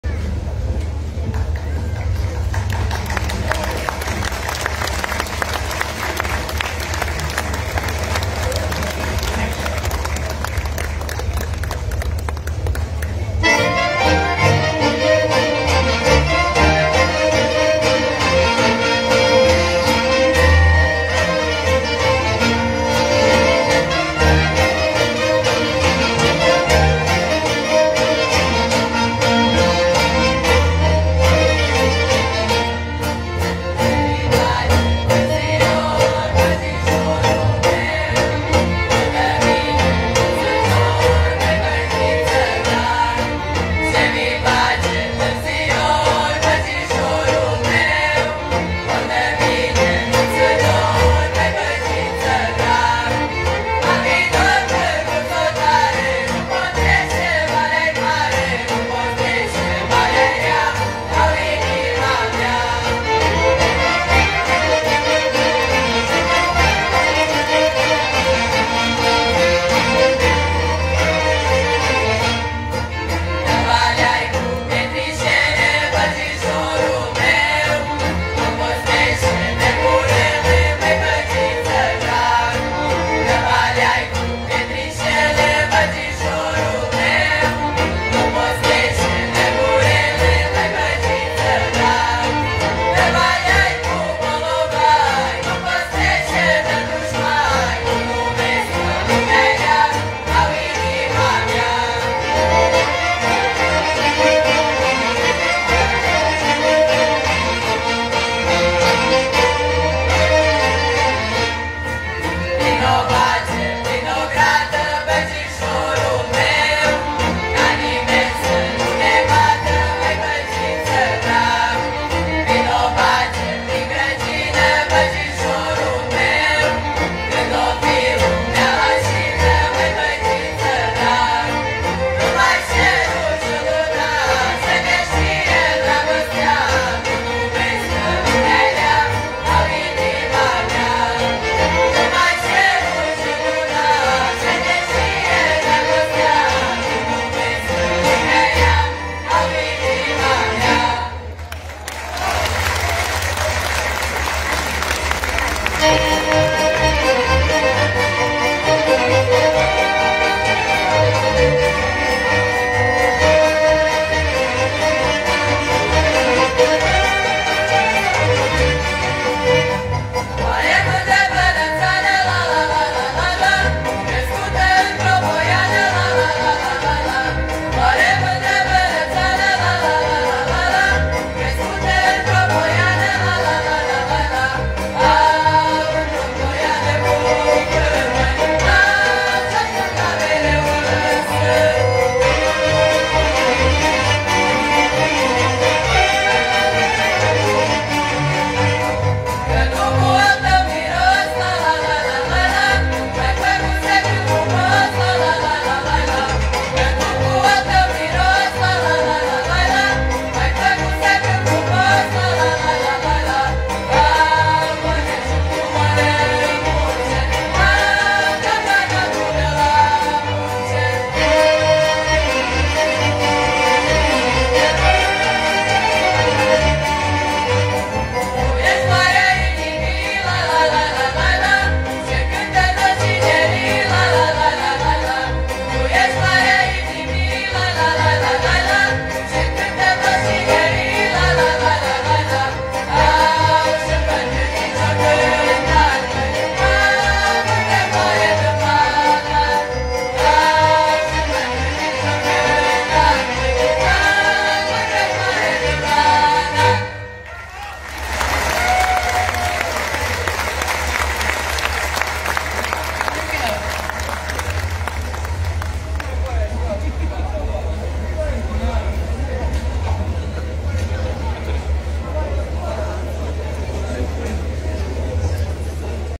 Grupul feminin "Casa tineretului" - Festivalul de folclor si muzica romaneasca, San-Mihai 2023
Grupul vocal feminin "Casa tineretului" - Festivalul  de folclor si muzica romaneasca din Voivodina - Republica Serbia, San Mihai 2023